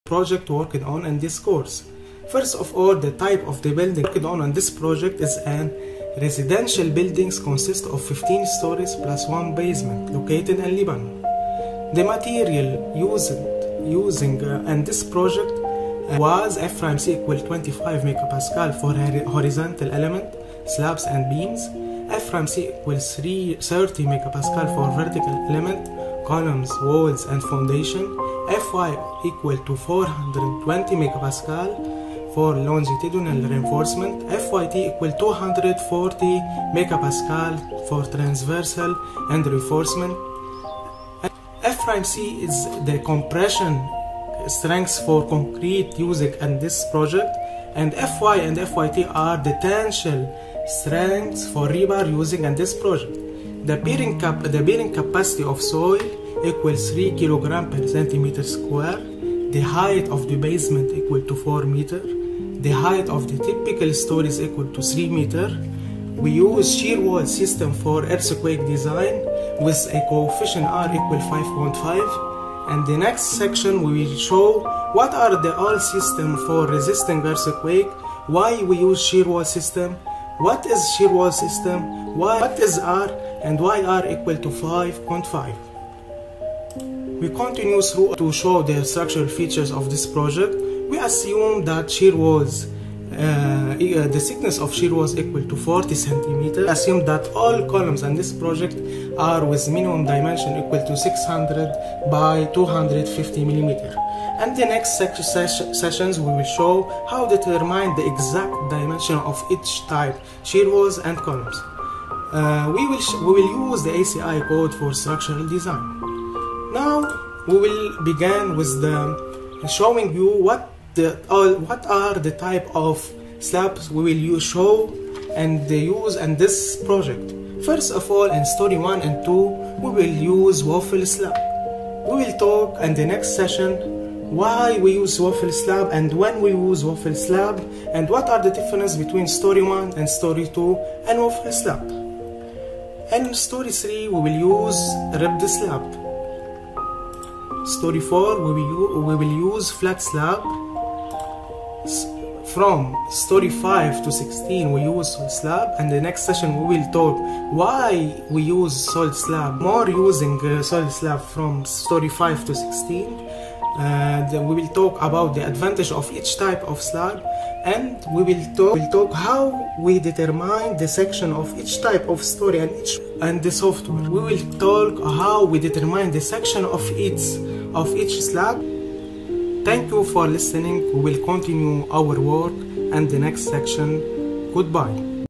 ETABS, SAFE, SAP2000 and ALLPILES in the structural design of 15 stories building Introduction 1
To show our professional courses and get certificate go to the links above